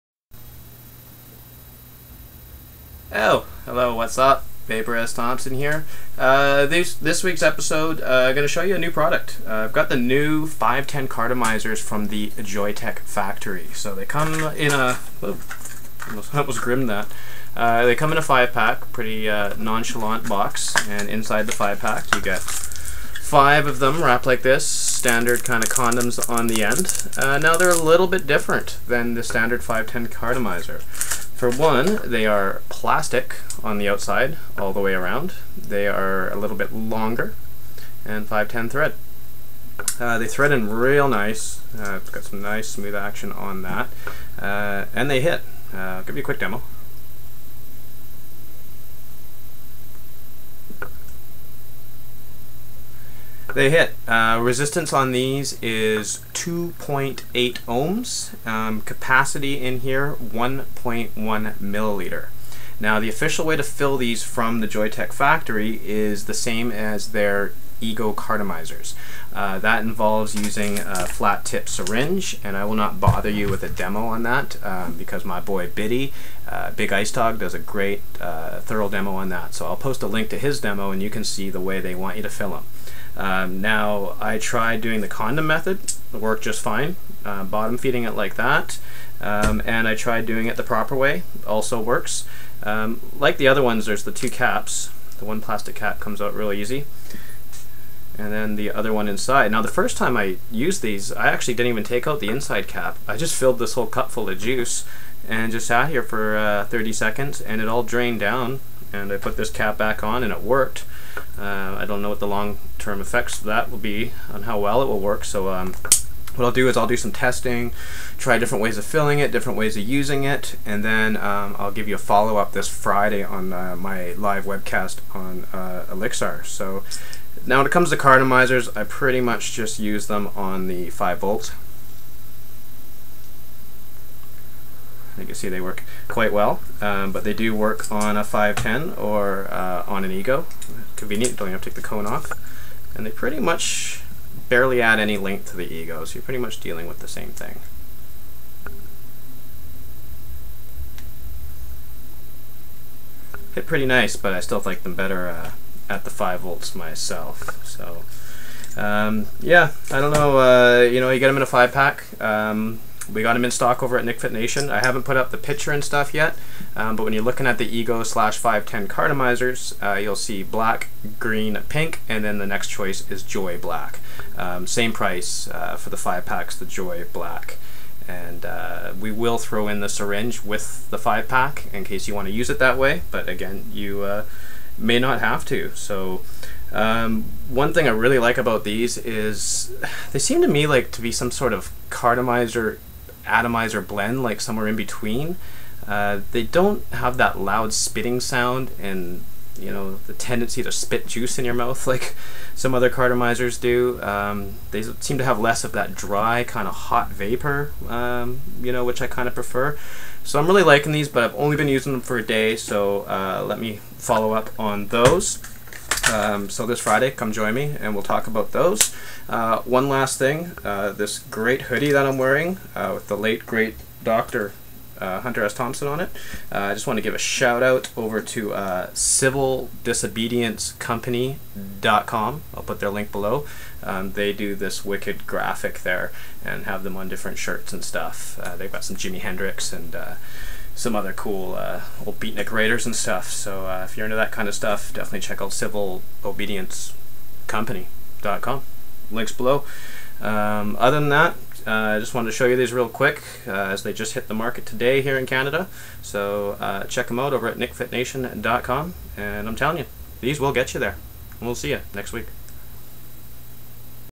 New 510 Joye Tech Carto Cartomizers review and demo
Here is the first look at the new 510 Cartomizer from the Joye Tech Factory. A little different vape that the regular 510 cartos. Seems like less heat and sputtering; feels like somewhere in between a carto and an atomizer.

Holds 1.1ml of e-liquid and has 2.8 ohm of resistance.

~Ship to Canada and USA only~

The Big Ice Dog video on how to fill these with a syringe: